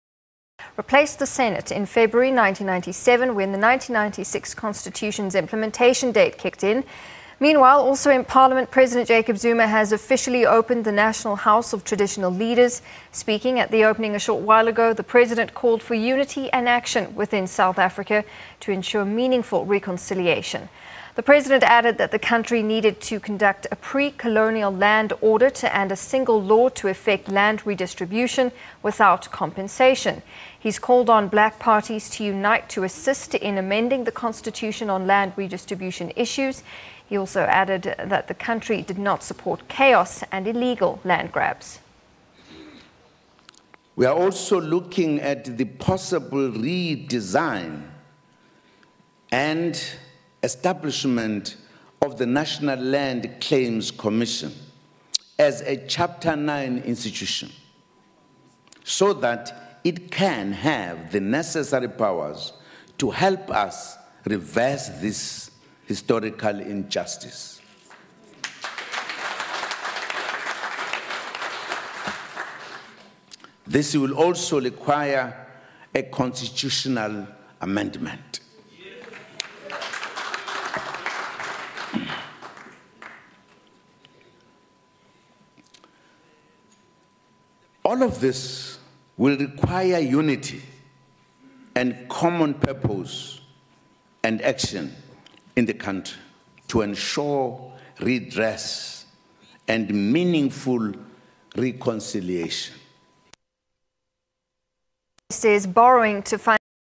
President Zuma officially opens Nat. House of Traditional Leaders
Parliament is on Friday marking the 20th anniversary of the National Council of Provinces.  The NCOP, the second chamber of Parliament, replaced the Senate in February 1997 when the 1996 consitution's implementation date kicked in. Meanwhile, also in Parliament, President Jacob Zuma has officially opened the National House of Traditional Leaders. Speaking at the opening, the President called for unity and action within South Africa to ensure meaningful reconciliation.